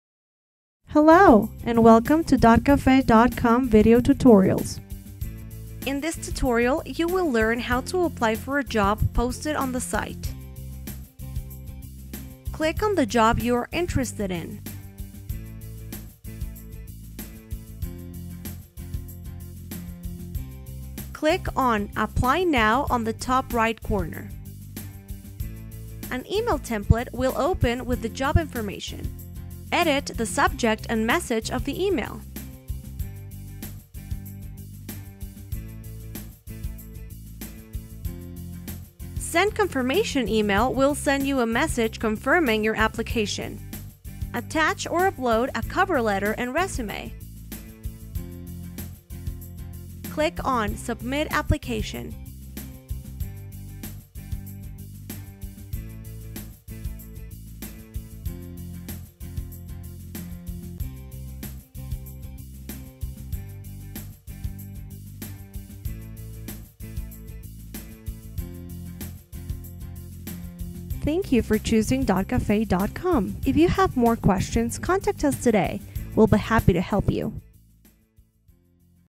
How do I apply for a job posted on DocCafe.com?
How do I apply for a job posted on the site?

1. When you’re viewing a job posting, click Apply Now. This will bring you to a screen to send the company an email to inquire about the job.
2. You’ll see a default email template you can use to send to the company to apply for the job, or you can draft your own email.
3. Attach a Cover Letter and/or Resume from one you have saved in your account or upload a new one.
4. Click Apply Now to send your email and apply for the job.